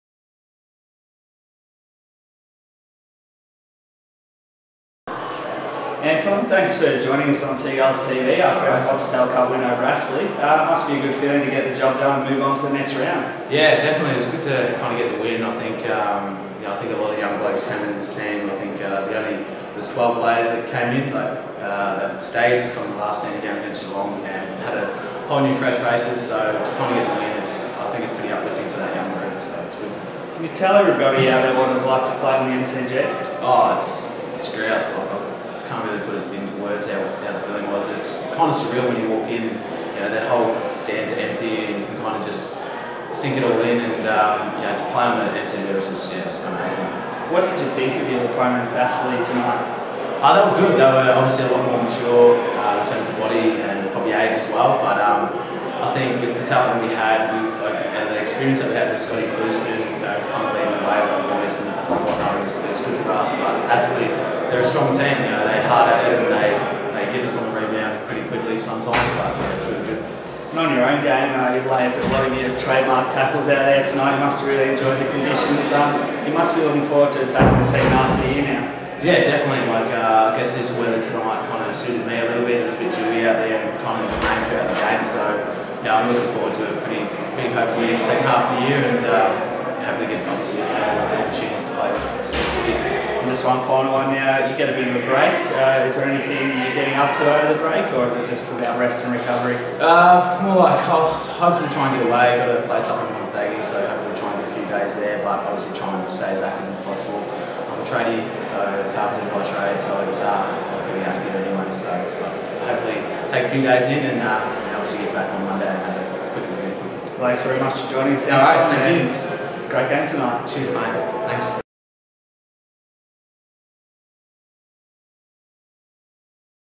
Anton Woods Interview - Foxtel Cup
We had a chat to Anton Woods about what it was like playing on the MCG after the club's 45-point win over Aspley in the Foxtel Cup.